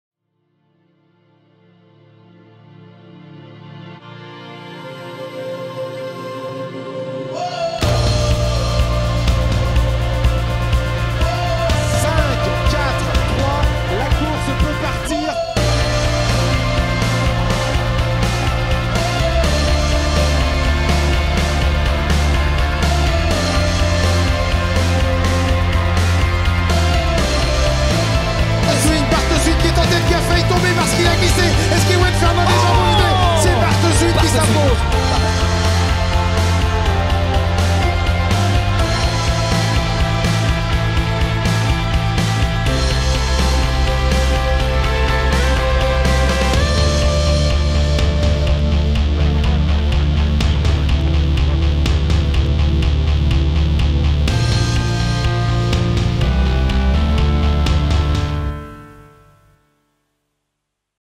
WORLD INLINE CUP Trailer 2017
Get ready for the 2017 WORLD INLINE CUP,  the international inline marathon series.